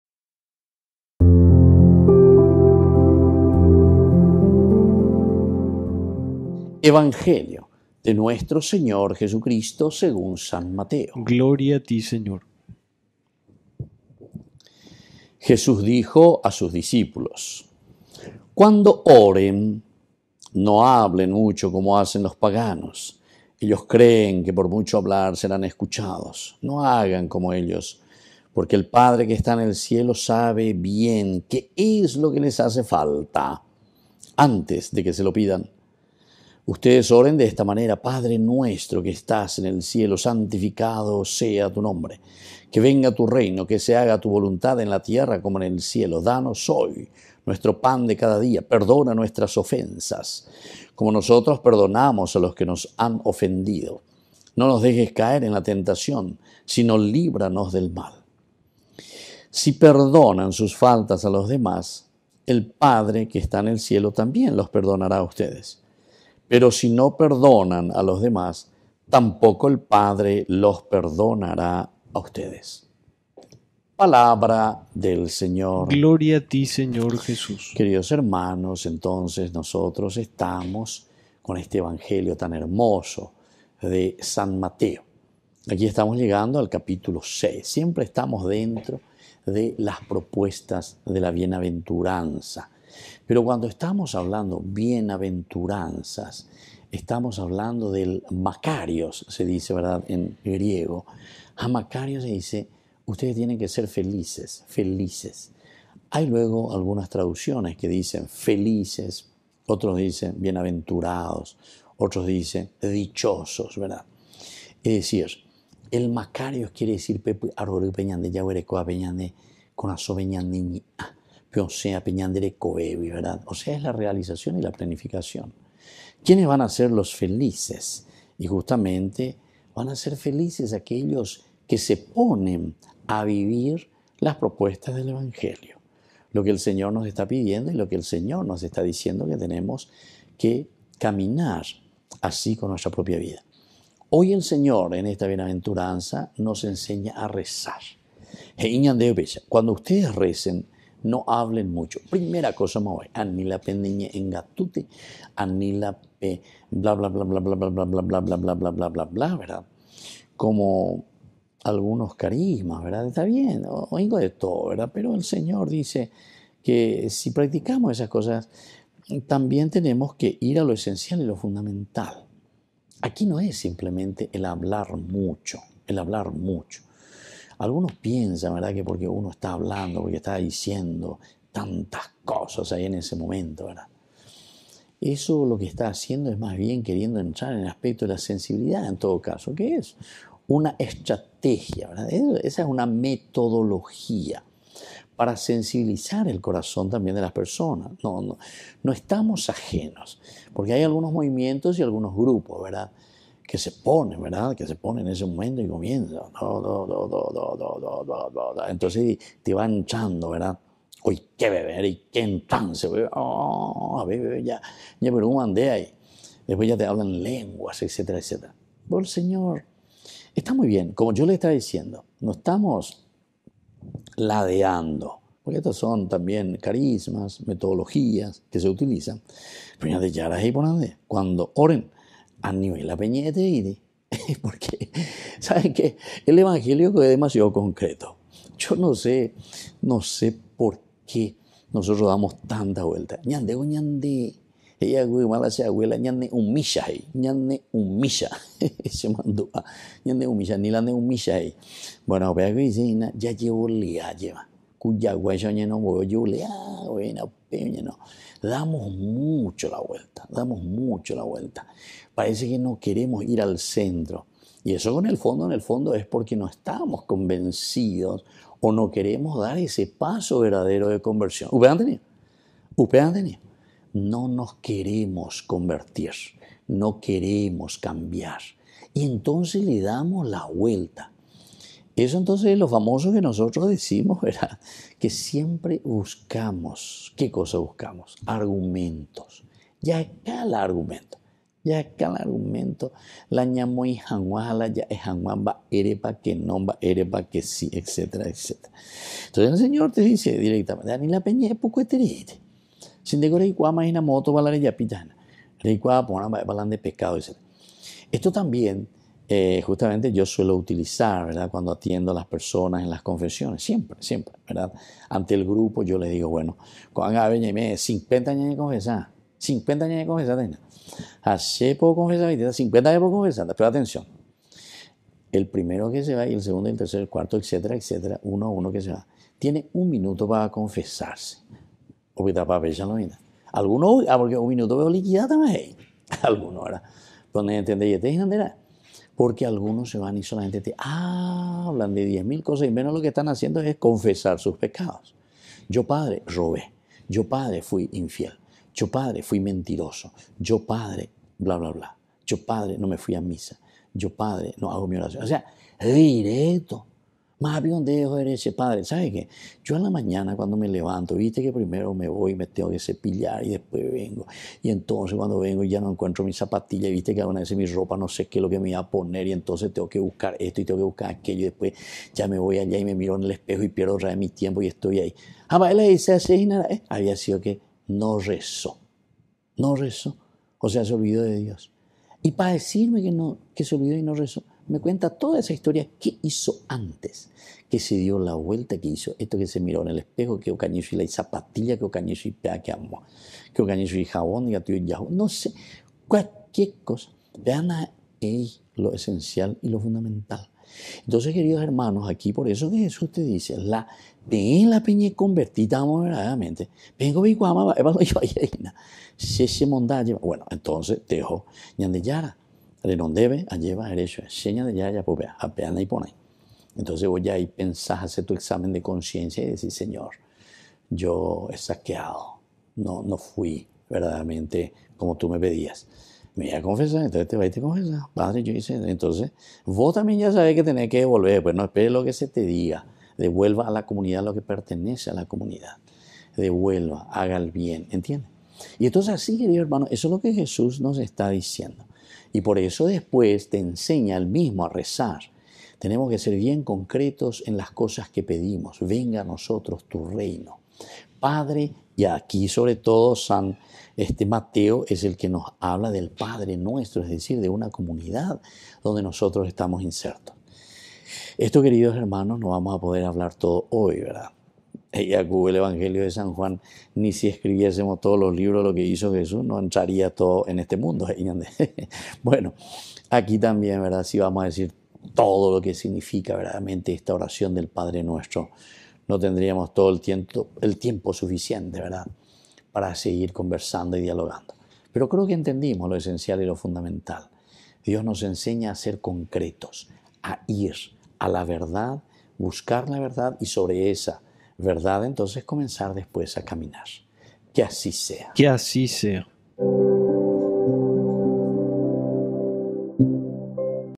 HOMILIA DE HOY | Jueves 22 06 23
Santo Evangelio según San Mateo 6, 7-15:
Jesús dijo a sus discípulos: “Cuando oren, no hablen mucho, como hacen los paganos.”